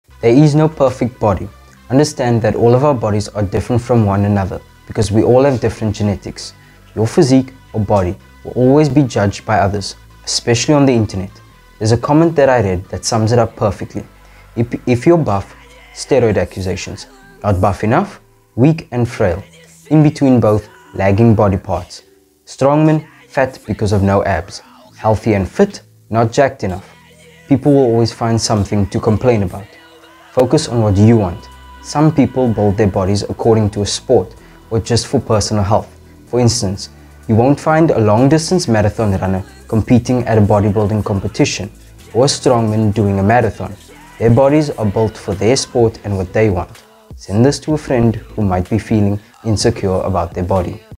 Uncovering the Truth Behind "The Perfect Body"
workout perfectbody aesthetic aestheticphysique muscle abs v-taper#thetruth#thesecret#marshfitness

There is no "Perfect body". Everybody is different and each person has different goals. Some people just want to be fast while others might want to be stronger in certain movement patterns. Train according to your goals, not other people's goals.